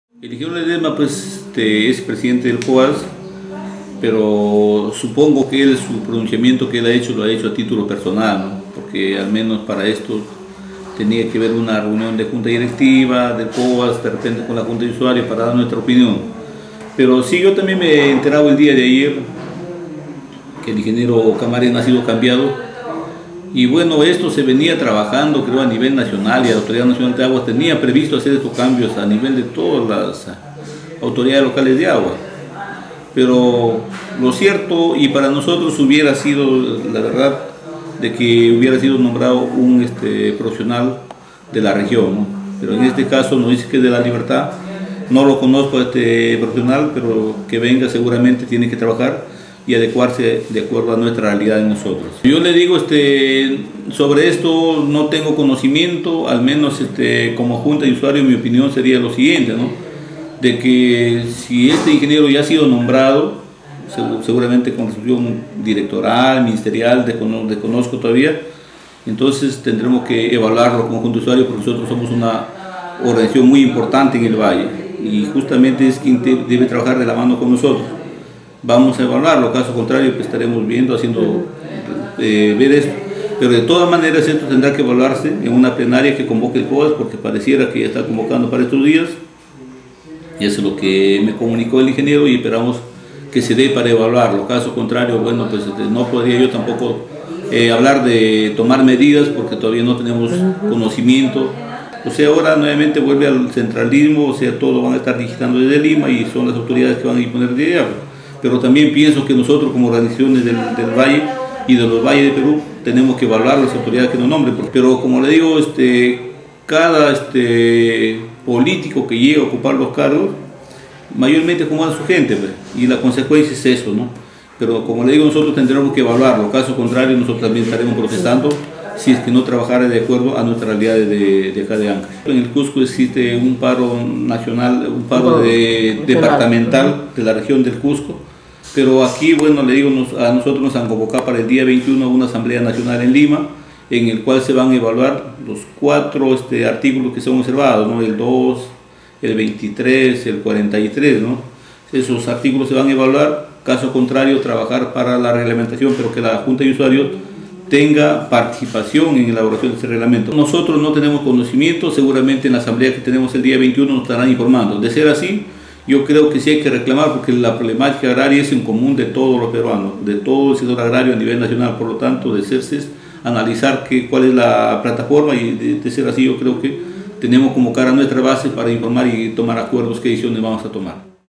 CANAL31 - PDTE JUNTA DE USUARIOS DEL SANTA - JULIO ROSSO - LAMENTO NOMBRAMIENTO DE TRUJILLANO
PDTE JUNTA DE USUARIOS DEL SANTA, JULIO ROSSO MANIFESTO SU IMPRESION SOBRE EL NOMBRAMIENTO DE UN TRUJILLANO AL CARGO REPRESENTATIVO EN LA JUNTA DE USUARIOS.